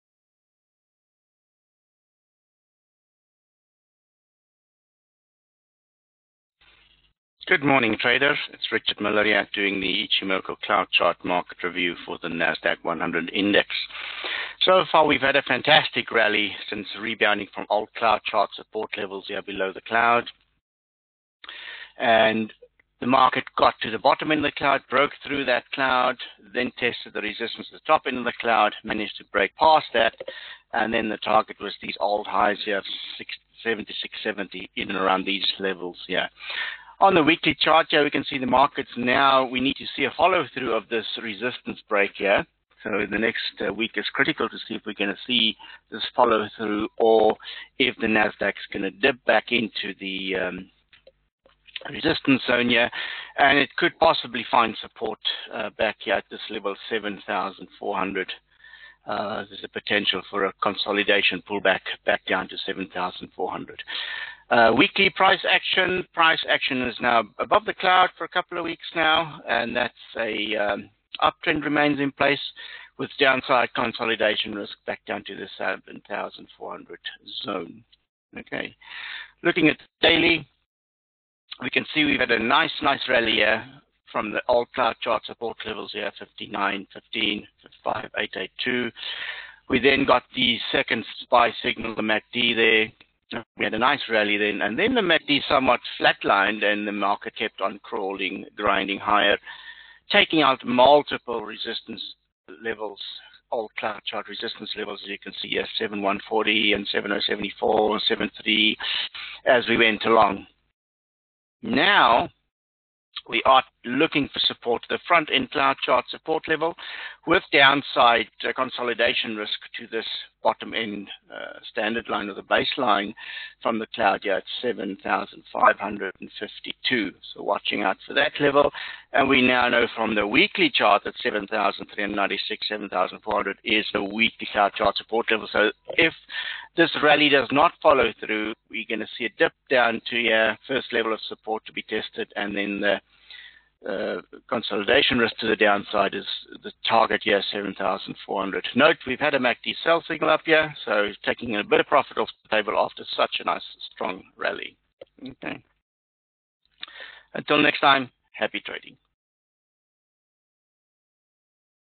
Nasdaq 100 Index: NDX Cloud chart trading market review.
NDX: Consolidation risk to key cloud support levels remain in place now as the index is navigating the old high resistance levels!